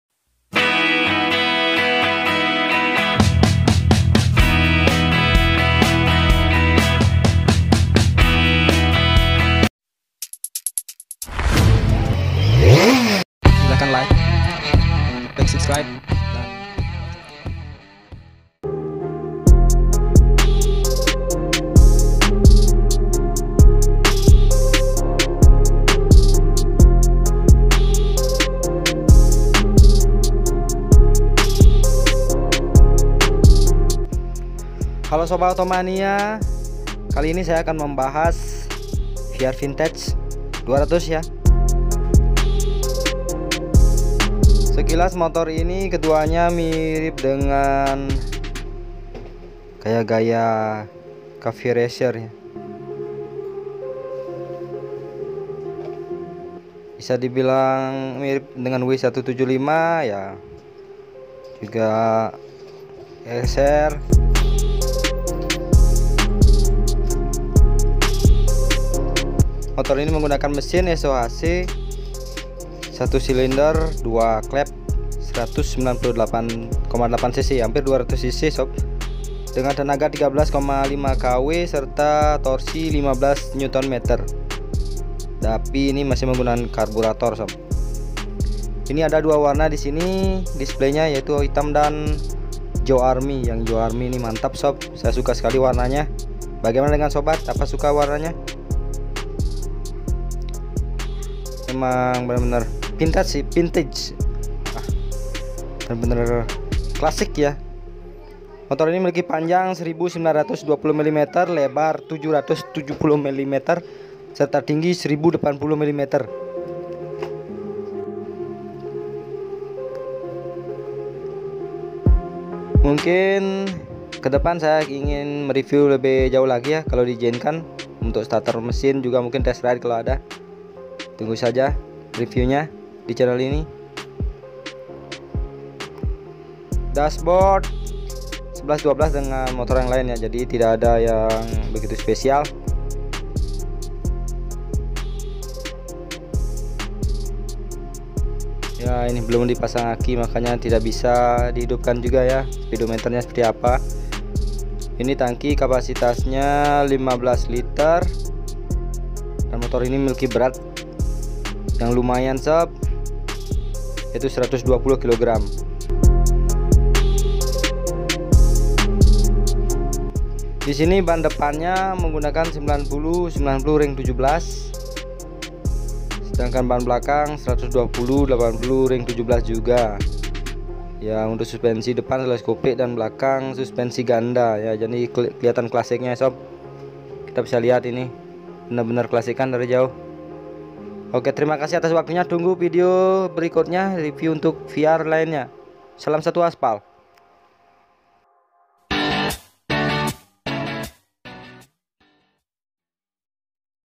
First Impression VIAR VINTECH 200 - dibilang Caferacer iya Vintage juga
First impression atau kesan pertama melihat Viar Vintech ini sangat keren. Sesuai gaya bikers kekinian yang suka dengan desain klasik berpadu cafe racer. Viar vintech ini pas buat biker bergaya retro namun bisa membeli dengan harga terjangkau. Vintech dijual mulai 22 jutaan dan sudah menggunakan mesin 1 silinder sohc 198cc 2 klep yang memiliki power 13,5 kw serta torsi 15nm. tunggu review tes ridenya ya untuk viar vintech 200 ini. Terimakasih